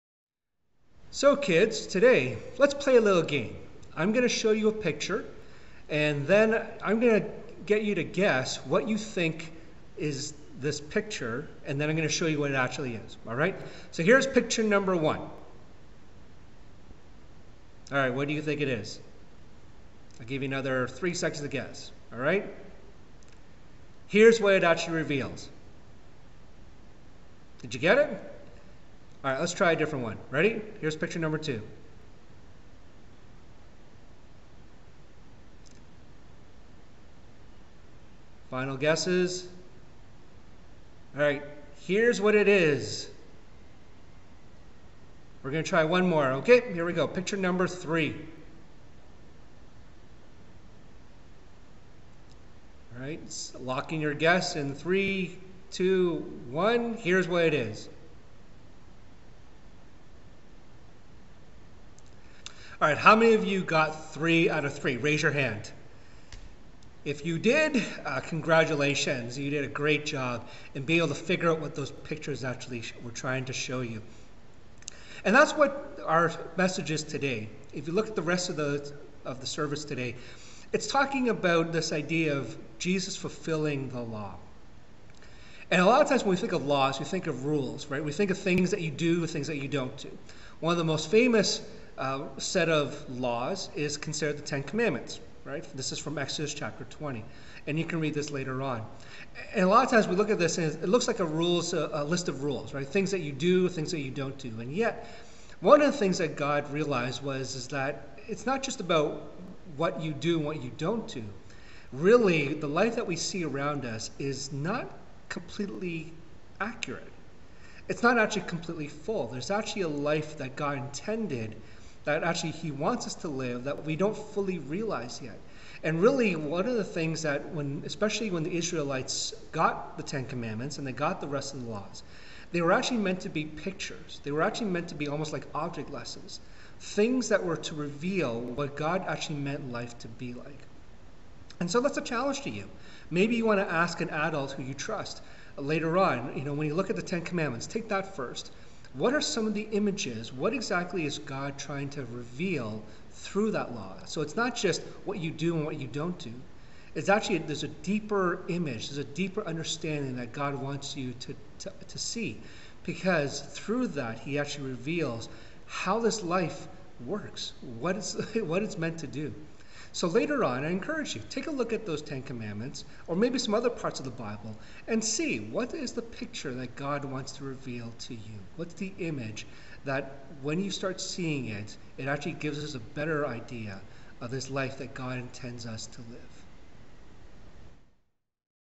MBC Children's Lesson: July 19, 2020
Alvin shares one way to view God's law with a game.